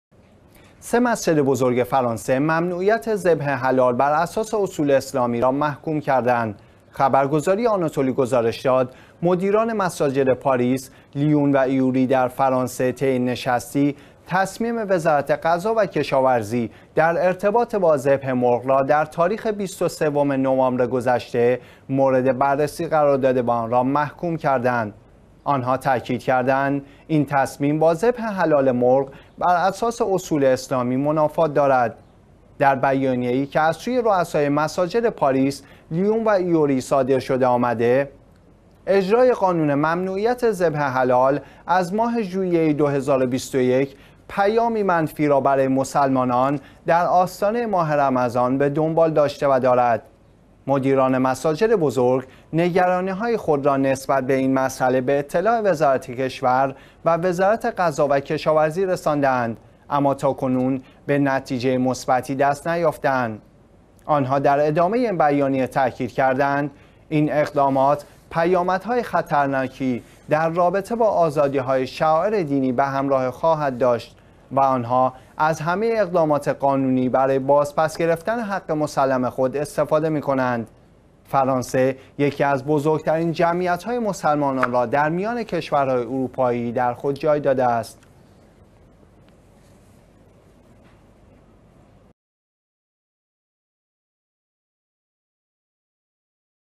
اعتراض مساجد بزرگ پاریس به ممنوعیت ذبح حلال
سه مسجد بزرگ فرانسه ممنوعیت ذبح حلال براساس اصول اسلامی را محکوم کردند.
خبرگزاری آناتولی گزارش داد: مدیران مساجد پاریس، لیون و ایوری در فرانسه طی نشستی، تصمیم وزارت غذا و کشاورزی در ارتباط با ذبح مرغ را در تاریخ ۲۳ نوامبر گذشته مورد بررسی قرار داده و آن را محکوم کردند.
آنها تأکید کردند: این تصمیم با ذبح حلال مرغ بر اساس اصول اسلامی منافات دارد.
در بیانیه‌ای که از سوی رؤسای مساجد پاریس، لیون و ایوری صادر شده، آمده: اجرای قانون ممنوعیت ذبح حلال از ماه ژوئیه ۲۰۲۱، پیامی منفی را برای مسلمانان در آستانه ماه رمضان به دنبال داشته و دارد.
مدیران مساجد بزرگ نگرانی‌های خود را نسبت به این مسئله به اطلاع وزارت کشور و وزارت غذا و کشاورزی رسانده‌اند اما تاکنون به نتیجه مثبتی دست نیافتند.
آنها در ادامه این بیانیه تأکید کردند: این اقدامات پیامدهای خطرناکی در رابطه با آزادی‌های شعائر دینی به همراه خواهد داشت و آنها از همه اقدامات قانونی برای بازپس گرفتن حق مسلم خود استفاده می‌کنند.
فرانسه یکی از بزرگترین جمعیت های مسلمان را در میان کشورهای اروپایی در خود جای داده است.